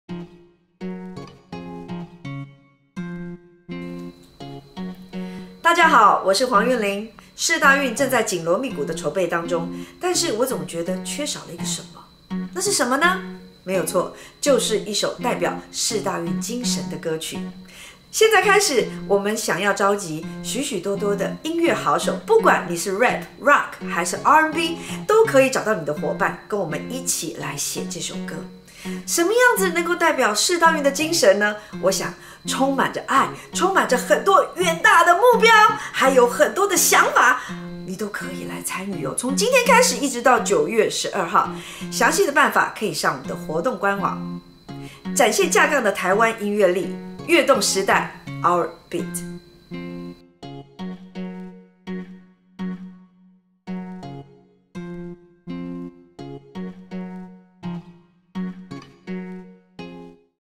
2017臺北世大運主題曲徵選活動評審召集人黃韻玲號召影片0712
邀請各方音樂好手一起來參與臺北世大運主題曲徵選活動，為國際運動盛會注入台灣豐沛的創作能量與活力。2017世大運「熱情、勇敢、夢想、愛」的精神，加上「運動、陽光」的心情，唱出臺灣獨特的魅力，讓世界聽見臺灣，讓臺北的聲音響遍全世界！樂動時代！Our Beat!